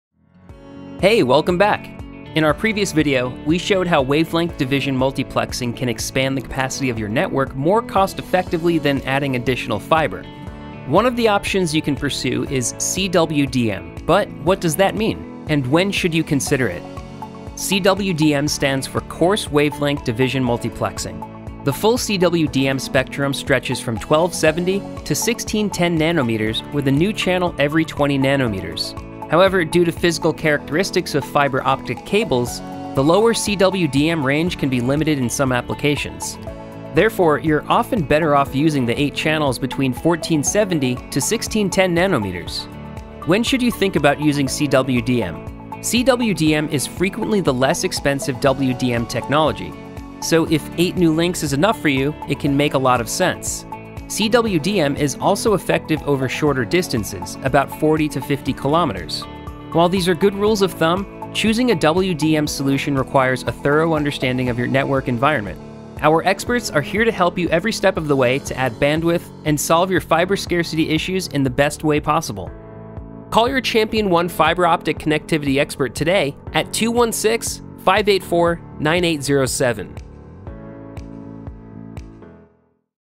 Coarse Wavelength Division Multiplexing (CWDM)
CWDM stands for coarse wavelength division multiplexing. The full CWDM spectrum stretches from 1270nm to 1610nm, with a new channel every 20nm. Learn about the limitations of CWDM and when to use it in your network.